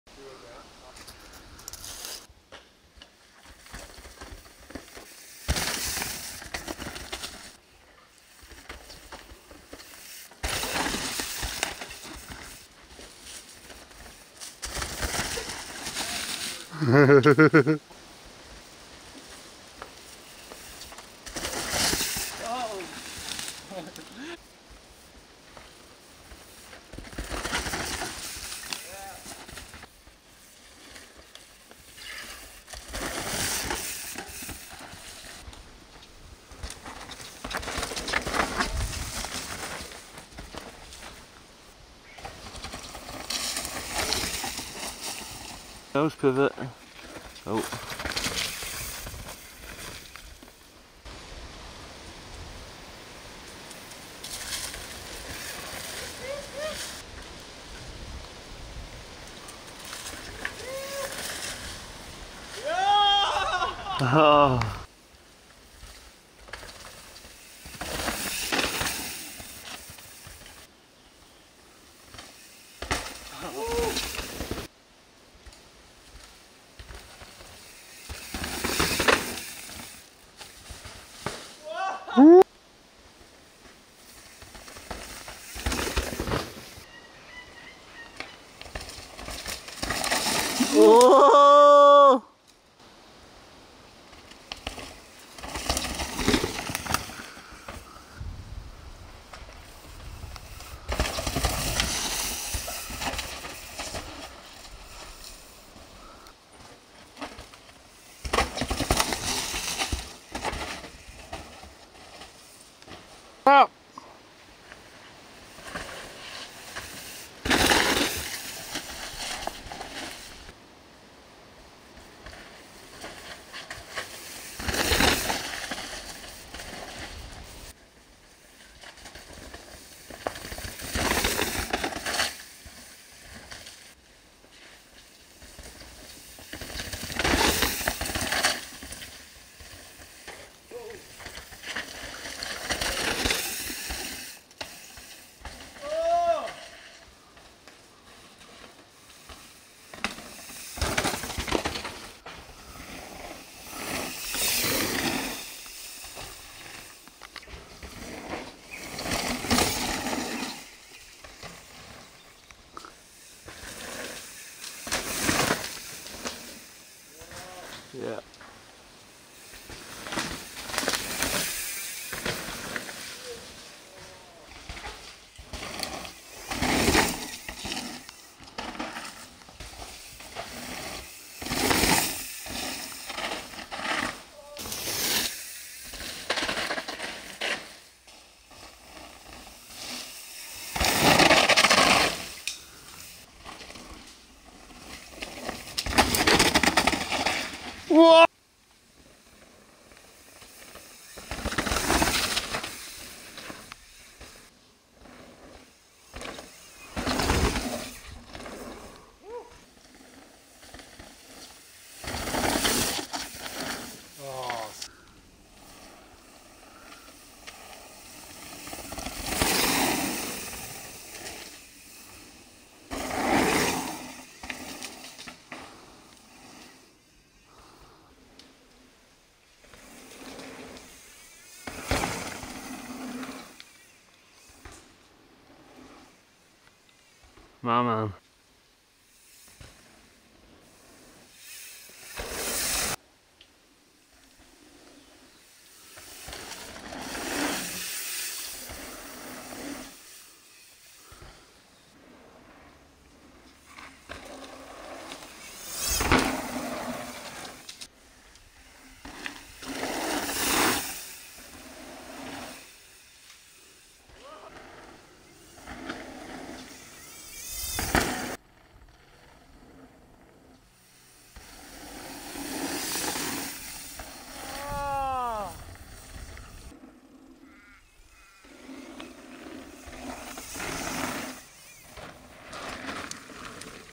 Jungle RAWr
About a year ago we visited Maydena Bike Park on the Australian island of Tasmania. Here's some of the footage we got in raw form. Riders: Sam Dale, Craig Evans, Josh Lewis.